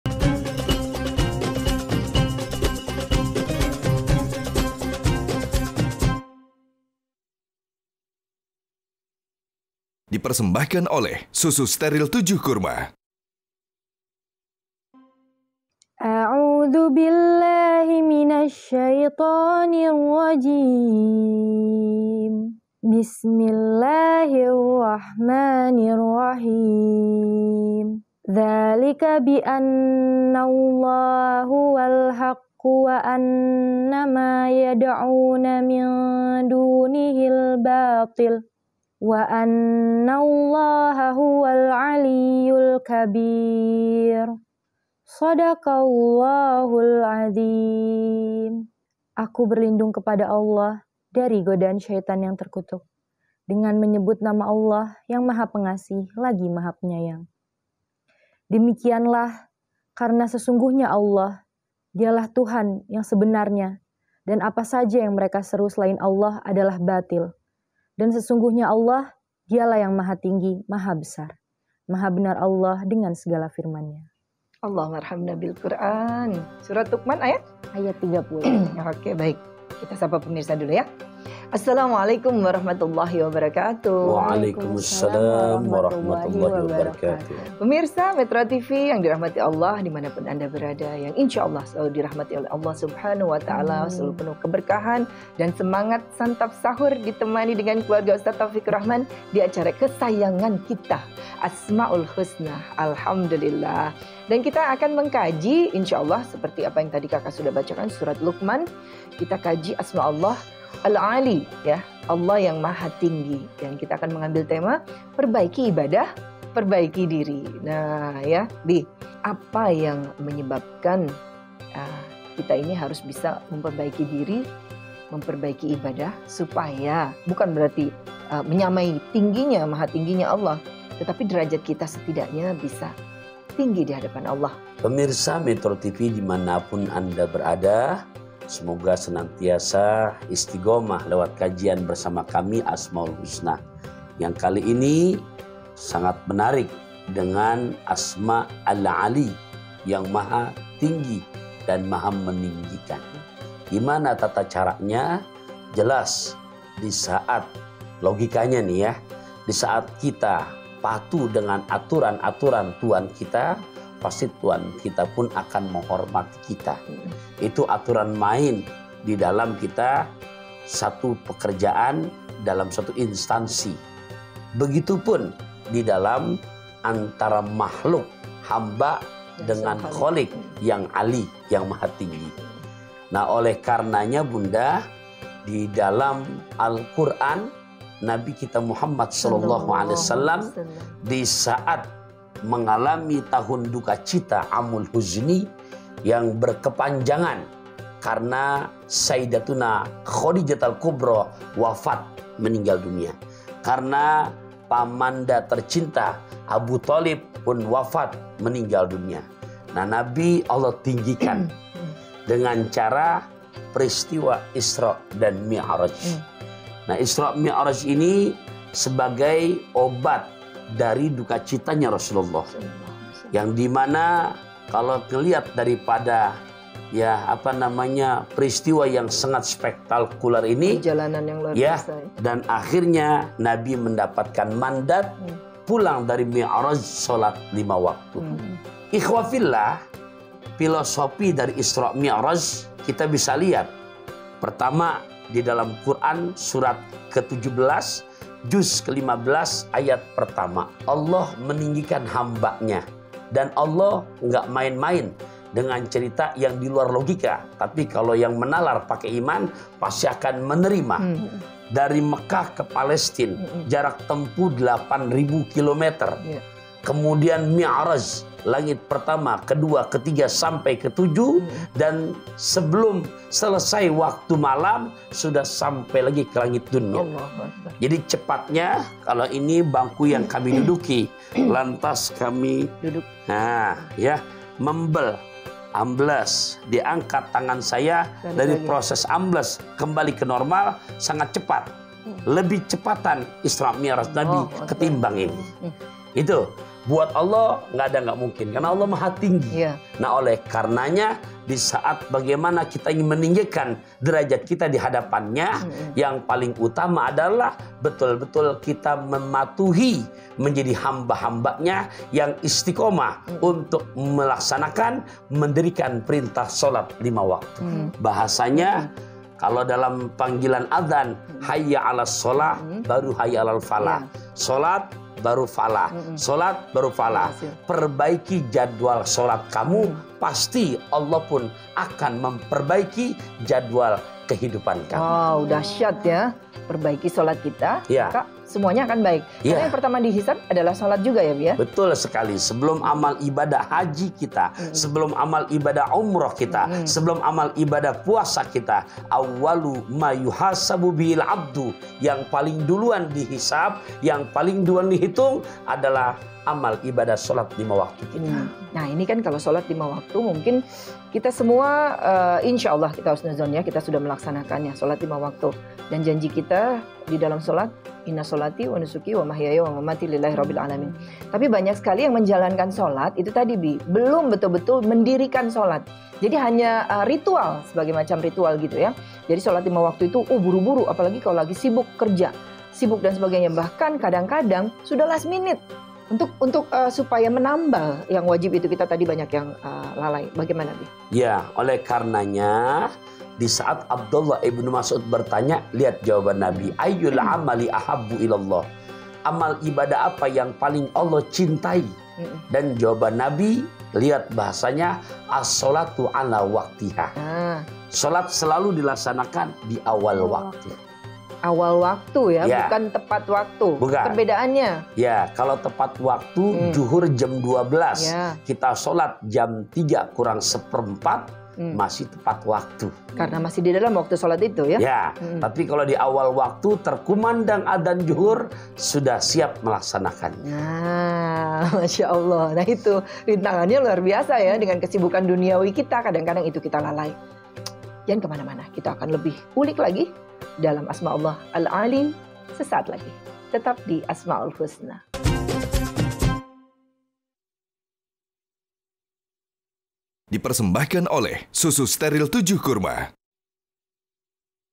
Asmaul Husna - Allah Yang Maha Tinggi
MetroTV, Taufiqurrahman dan keluarga hadir menemani sahur kalian dengan memberikan pemahaman terkait tafsir dan dakwah mengenai 99 nama Allah SWT.